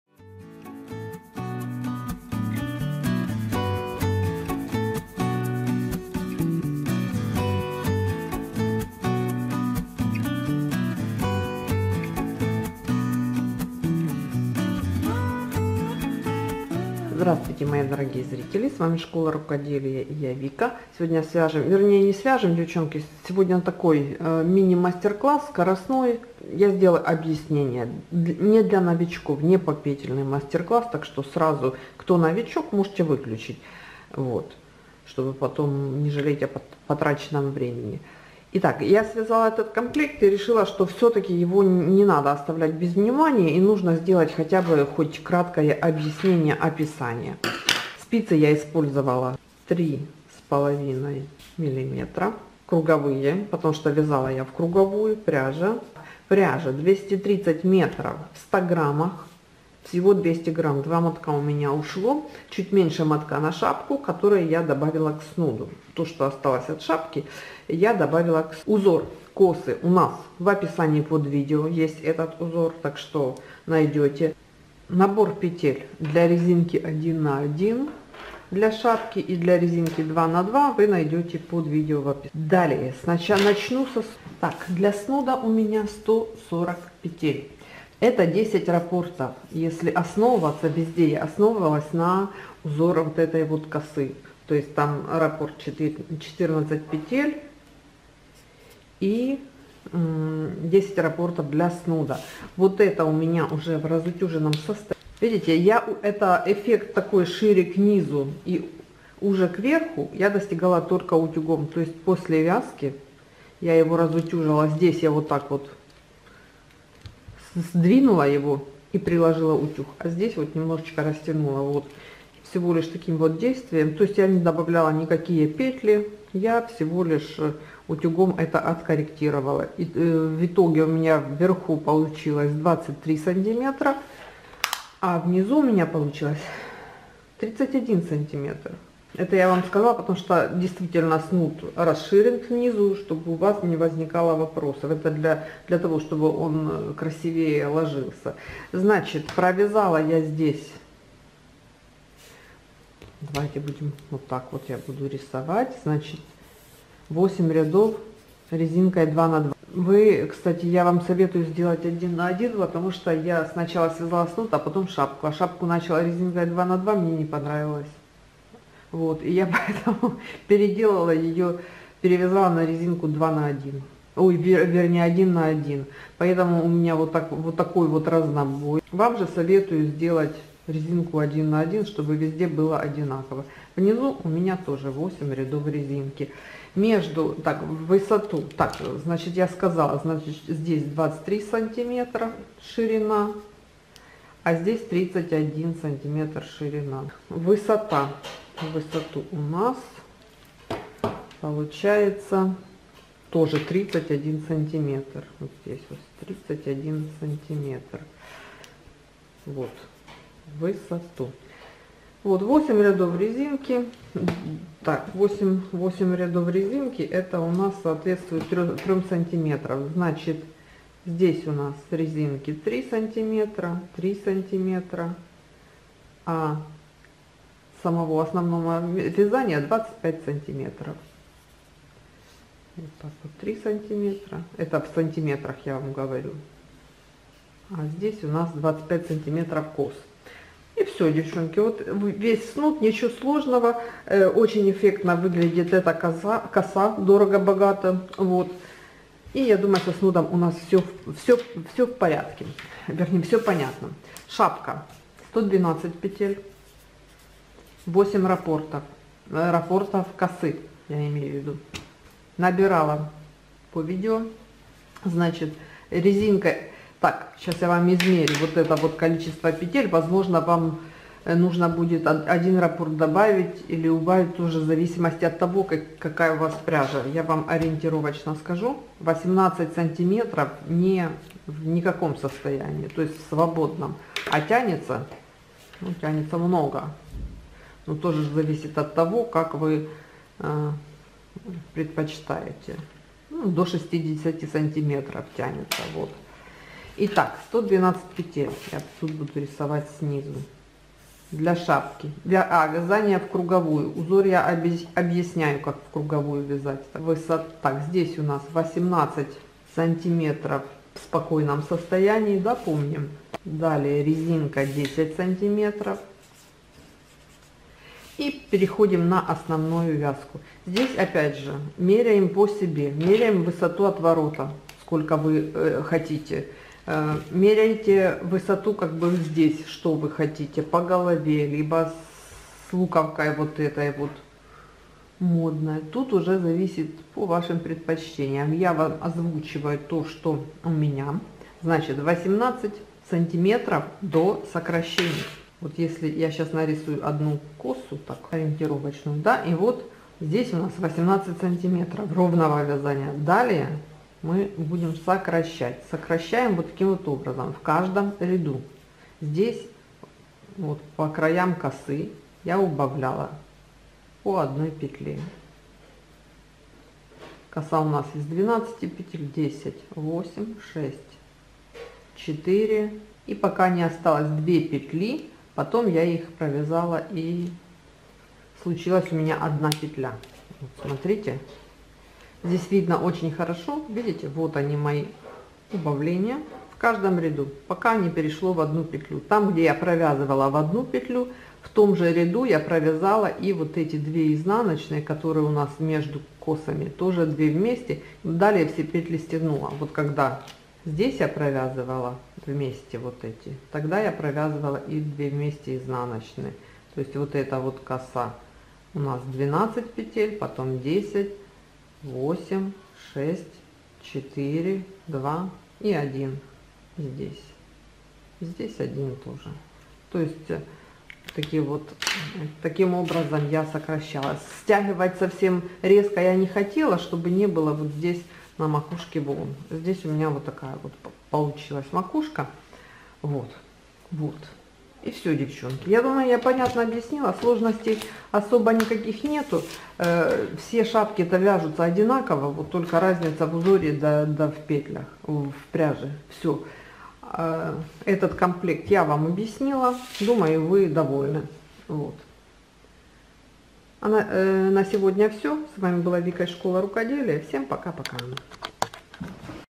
ШАПКА И СНУД С КОСАМИ.ШАПКА-БИНИ СПИЦАМИ.СНУД-ТРУБА С КОСАМИ.ОПИСАНИЕ ВЯЗАНИЯ КОМПЛЕКТА.
УРОКИ ВЯЗАНИЯ КРЮЧКОМ
kniting шапка спицами модное вязание